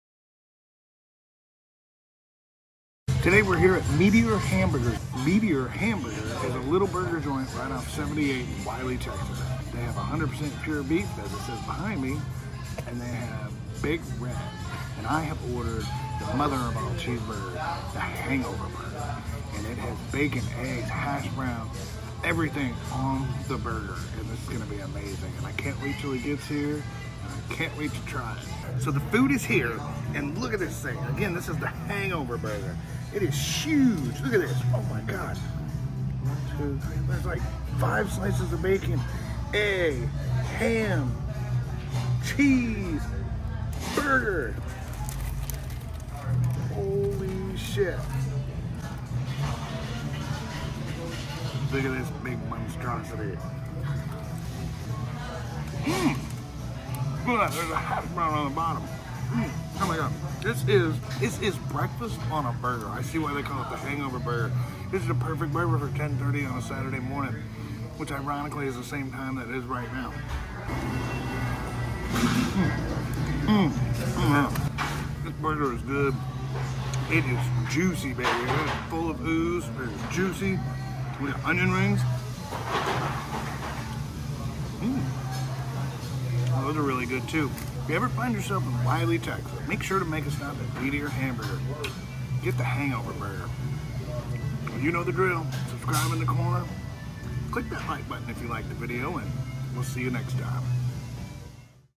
Brickeats Meteor Hamburger
Brickeats The Hangover Burger Meteor Hamburger in Wylie, Texas. Its a cheeseburger with hash browns, egg, bacon and ham. Its a mouthful for sure!!!